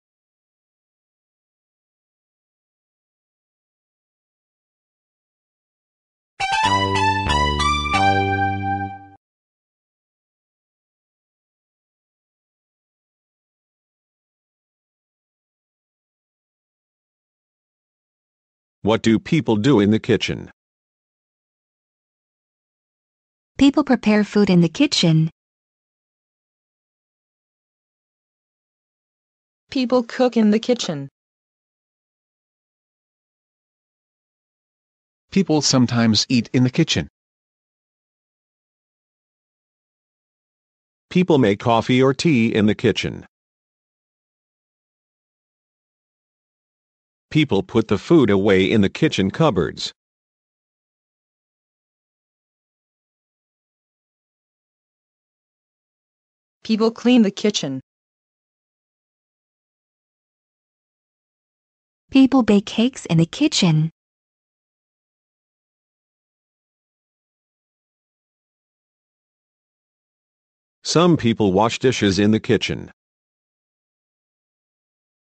Learning Vocabulary Video: Things to do in the kitchen
Từ vựng tiếng anh về hành động trong nhà bếp. Để xem chi tiết về phiên âm, nghĩa từ vựng; xin truy cập